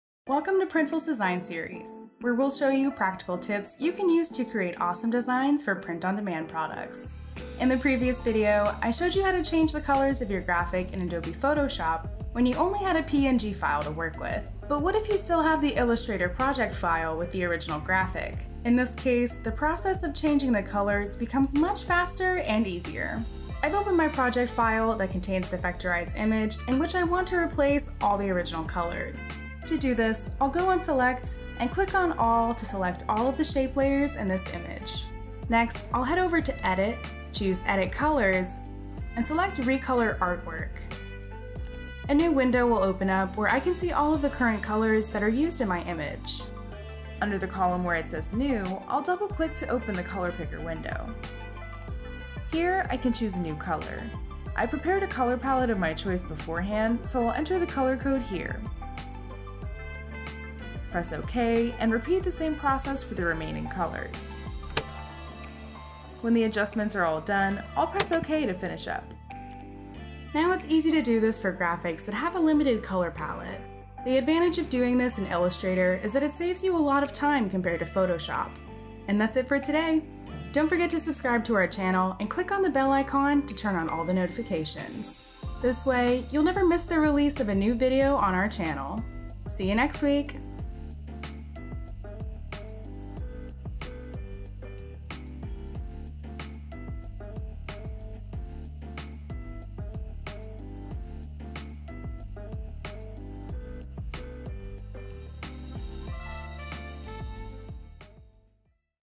Change Colors of Your Design in Adobe Illustrator: Printful Design Series
Sometimes great design is just a color (change) away! 🖼️ 🧑‍🎨
If you design vector graphics, take a look at our latest Printful Design series episode for quick tips on changing colors in your designs with Adobe Illustrator.

Check our Covid-19 FAQ page for the latest updates and fulfillment estimates ➡️

Monday to Friday from 9am to 8pm EST